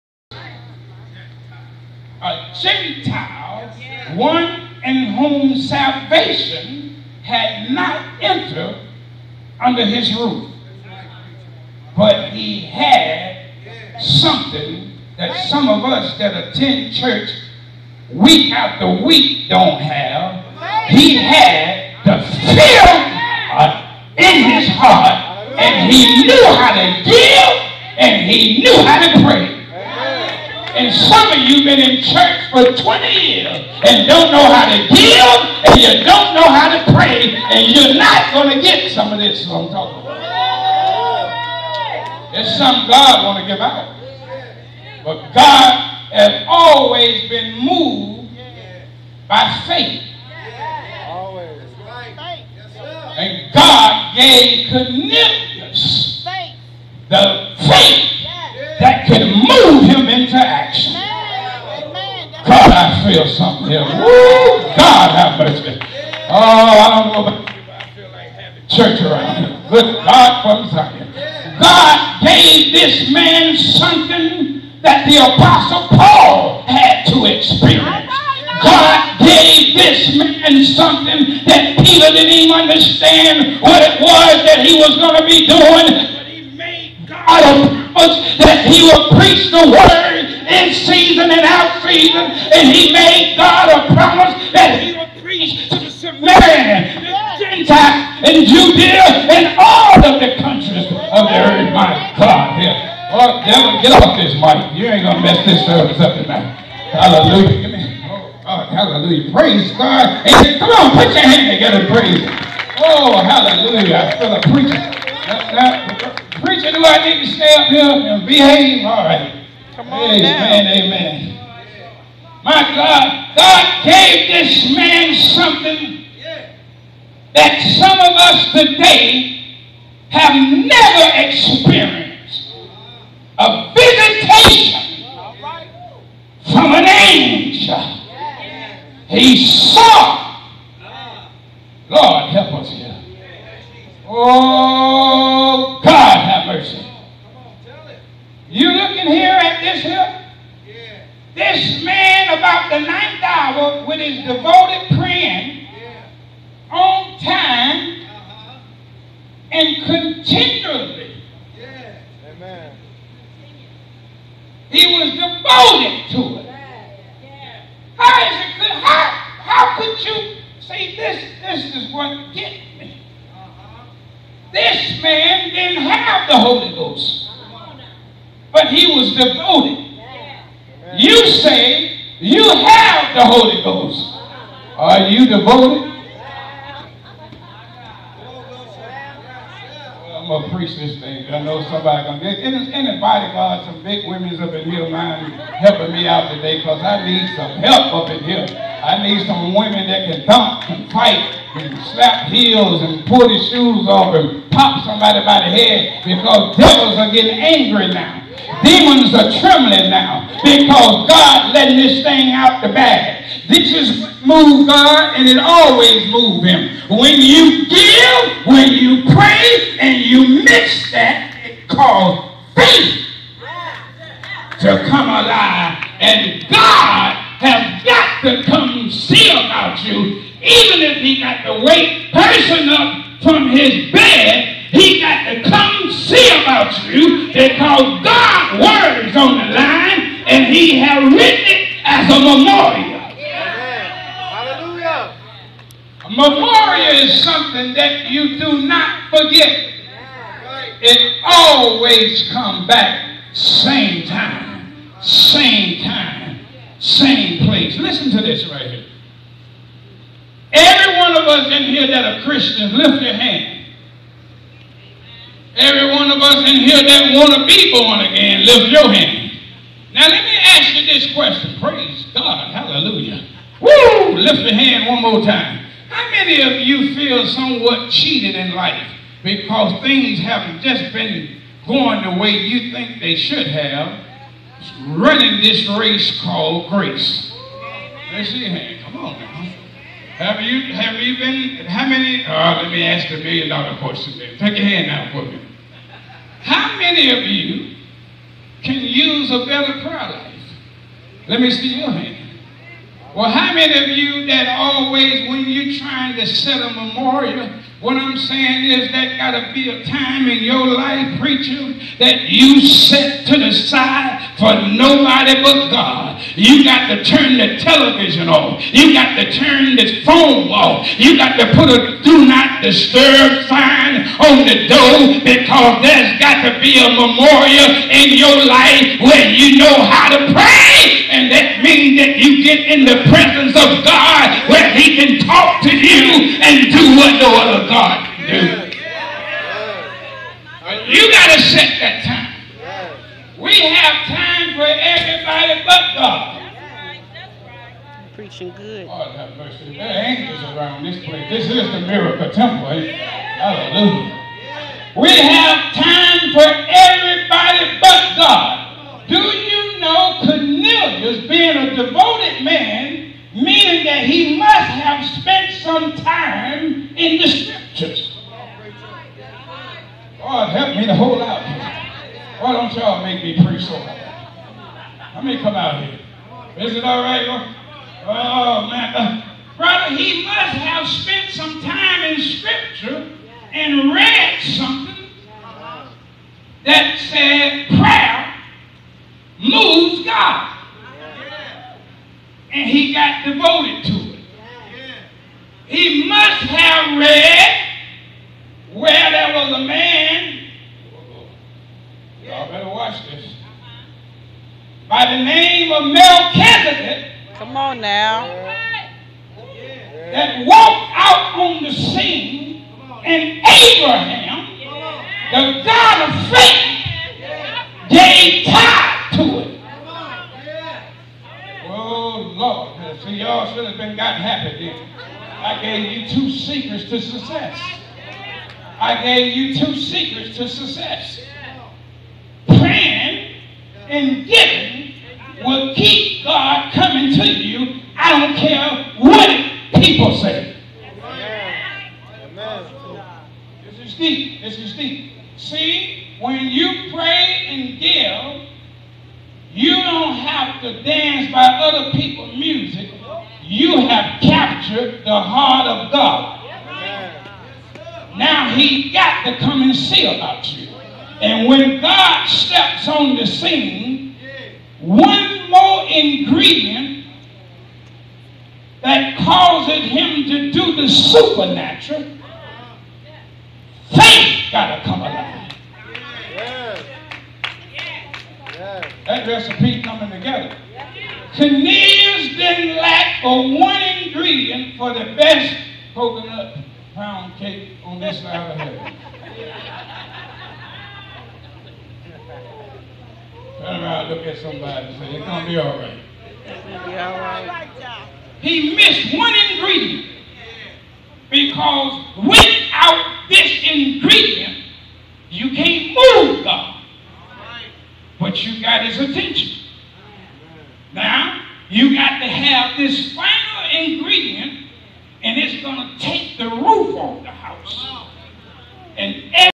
CORNER STONE CHURCH OF CHICAGO
Sunday July 27  2014  Anniversary Celebration for Apostle Cleveland R. Person